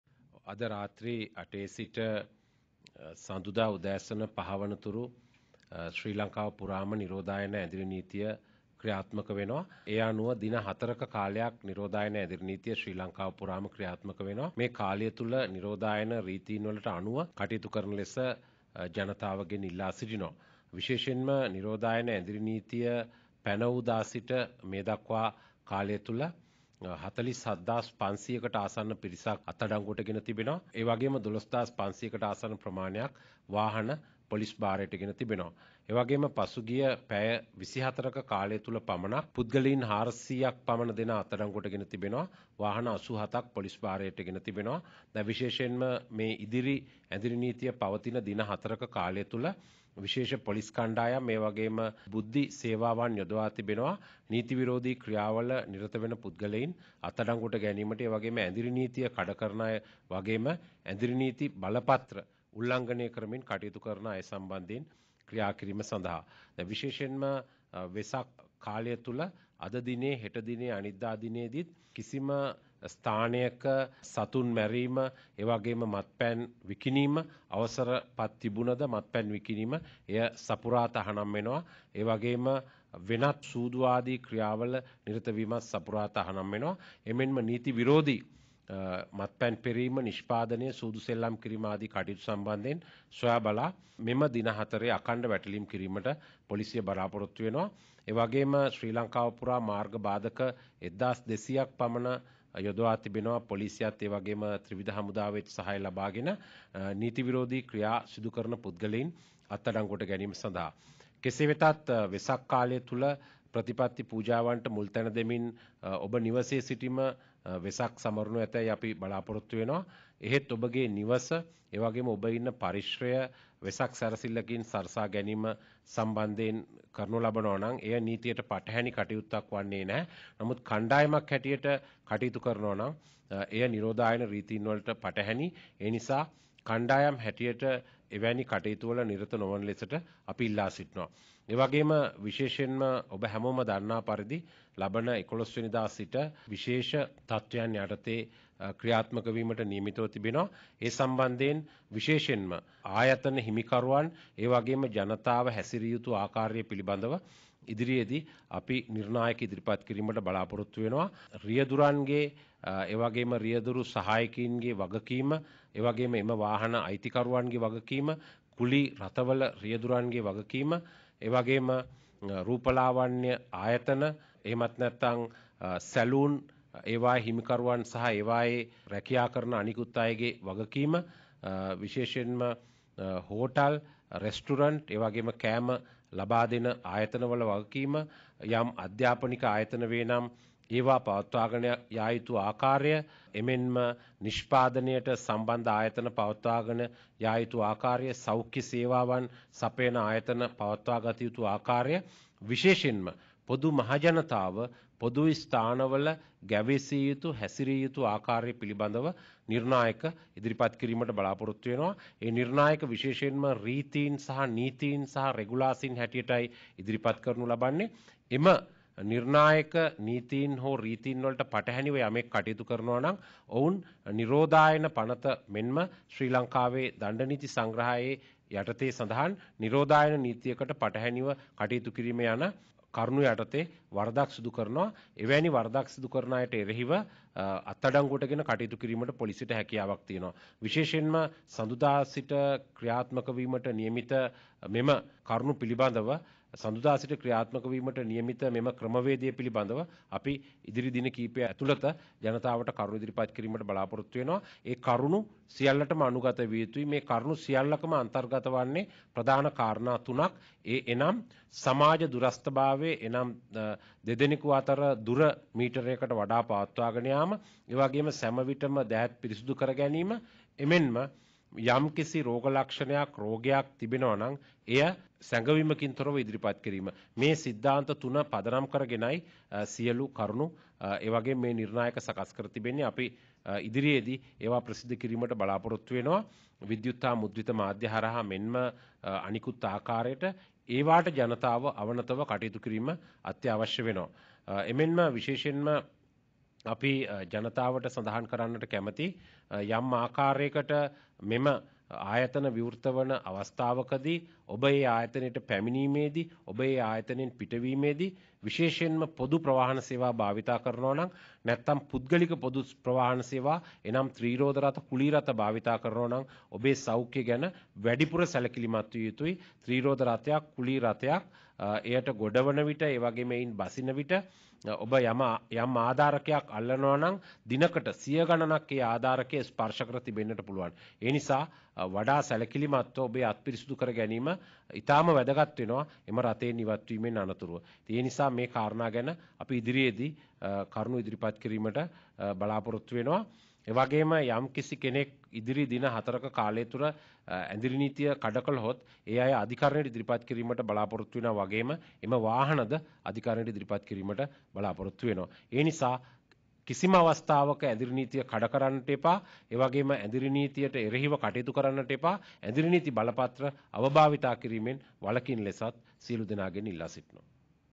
වෙසක් සමයේ හදිසි මෙහෙයුම්
මේ වෙසක් සතිය වෙනදා වගේ ගත කරන්න බෑ.. ඒ නිසා නිවාස තුළ ම ඉඳන් සමූහ වශයෙන් රැස් නොවී වෙසක් සමරමු! හදිසි මාර්ග බාධක වගේ ම පොලීසියේ විශේෂ මෙහෙයුම් රැසක් ක්‍රියාත්මකයි!